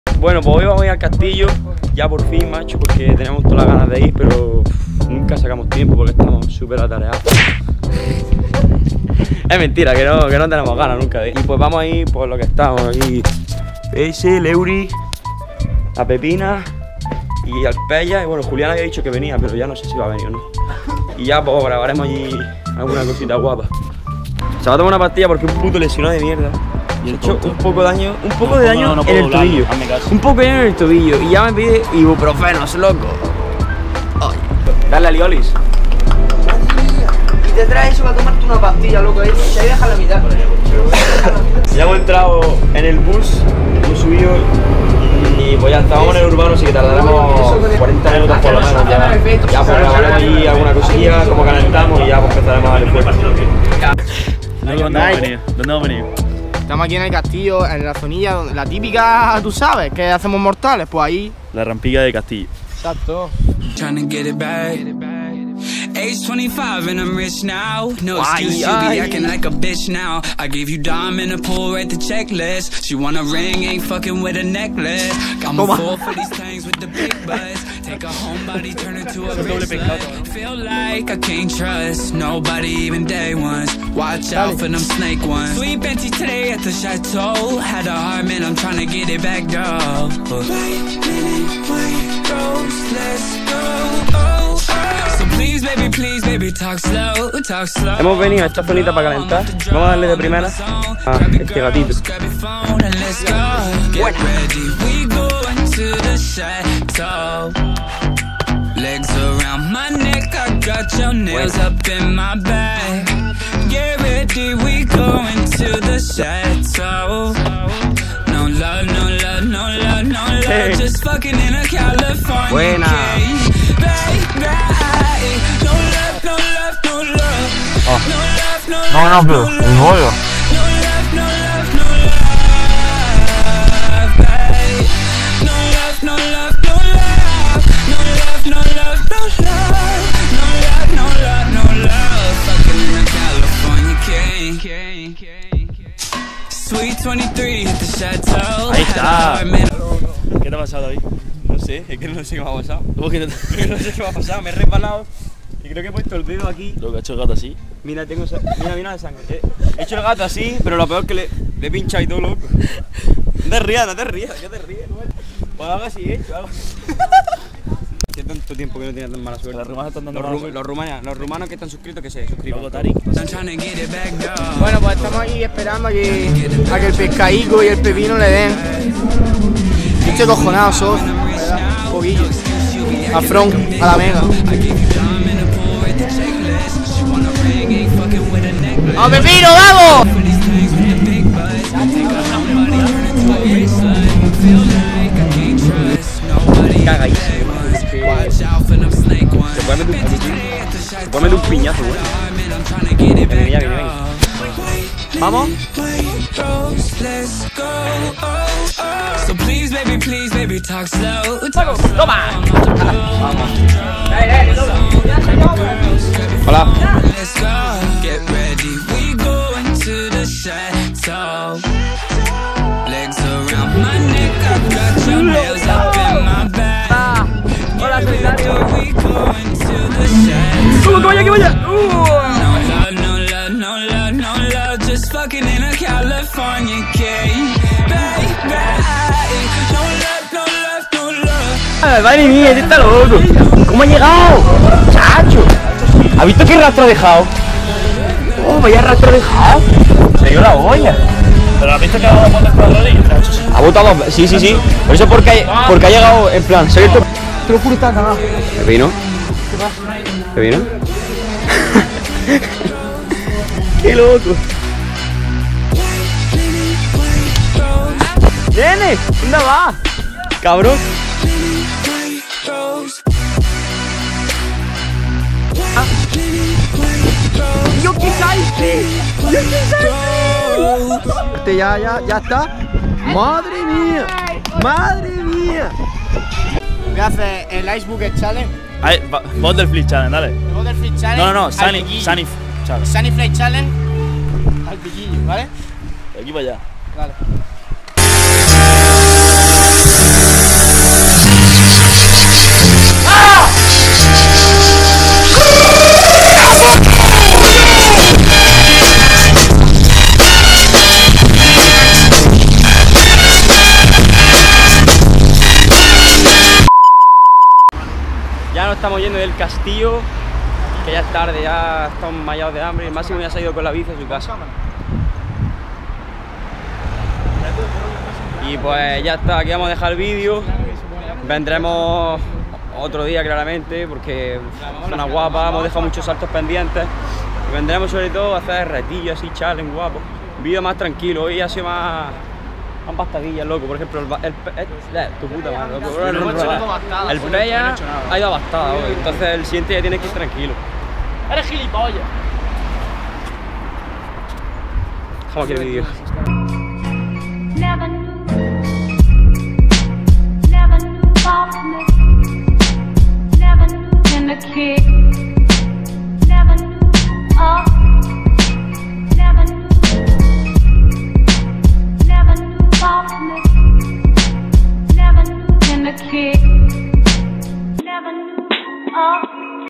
Entrenamiento por el CASTILLO de SANTA ANA, ROQUETAS
Cogemos el bus hacia Roquetas que está bastante cerca y echamos la mañana en el famoso Castillo de Santa Ana, una zona buenísima para hacer parkour.